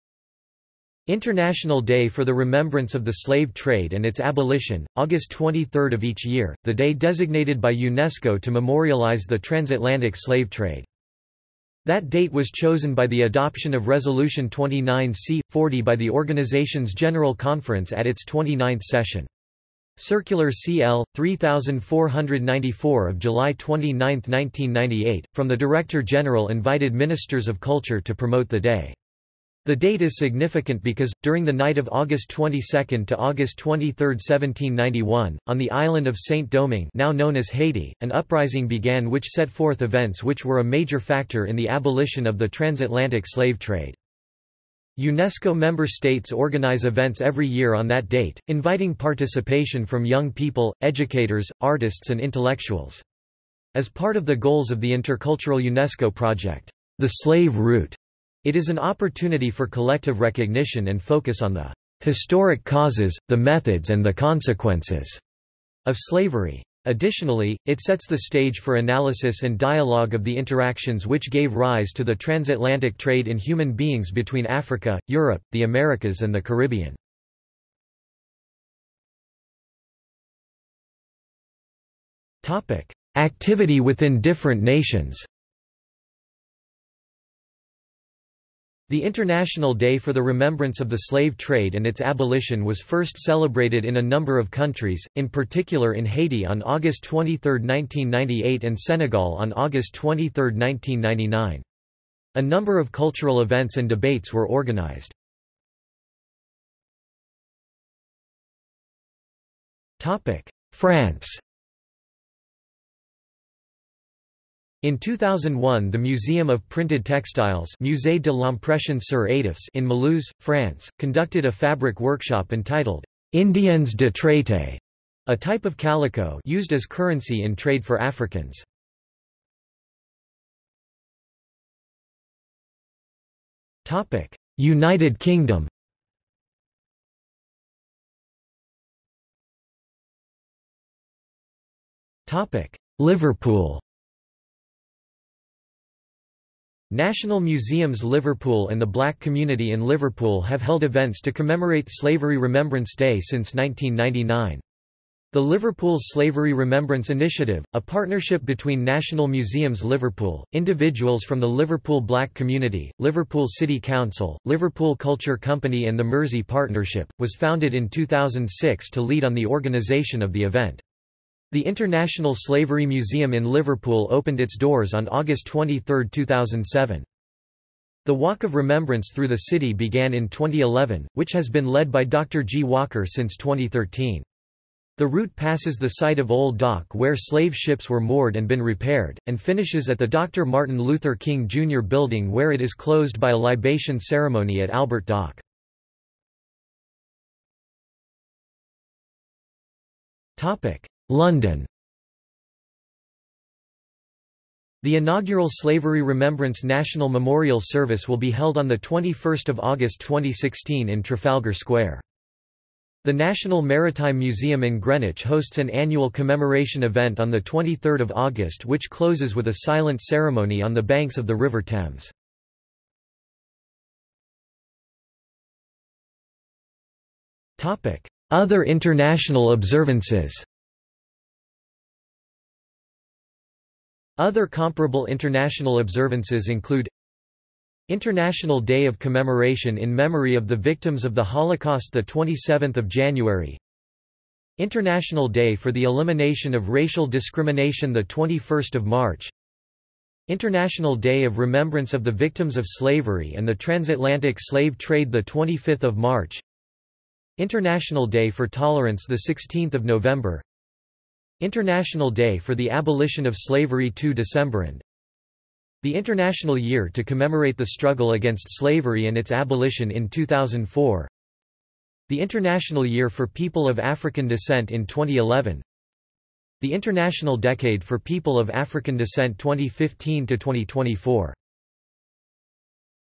International Day for the Remembrance of the Slave Trade and its Abolition | Wikipedia audio article
00:01:18 1 Activity within different nations
00:01:46 1.1 France
00:02:10 1.2 United Kingdom
00:02:19 1.2.1 Liverpool
00:03:17 1.2.2 London
00:03:44 2 Other international observances

- Socrates

SUMMARY
International Day for the Remembrance of the Slave Trade and its Abolition, August 23 of each year, the day designated by UNESCO to memorialize the transatlantic slave trade.
That date was chosen by the adoption of resolution 29 C/40 by the Organization's General Conference at its 29th session. Circular CL/3494 of July 29, 1998, from the Director-General invited Ministers of Culture to promote the day. The date is significant because, during the night of August 22 to August 23, 1791, on the island of Saint Domingue (now known as Haiti), an uprising began which set forth events which were a major factor in the abolition of the transatlantic slave trade.
UNESCO Member States organize events every year on that date, inviting  participation from young people, educators, artists and intellectuals. As part of the goals of the intercultural UNESCO project, "The Slave Route", it is an opportunity for collective recognition and focus on the "historic causes, the methods and the consequences" of slavery. Additionally, it sets the stage for analysis and dialogue of the interactions which gave rise to the transatlantic trade in human beings between Africa, Europe, the Americas and the Caribbean.